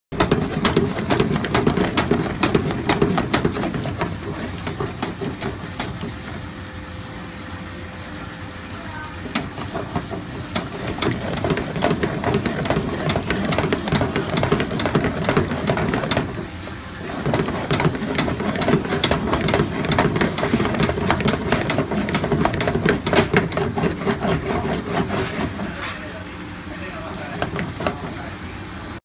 Restaurant Nu, sito manzano
Preparación del bacalao, para hacer brandada!!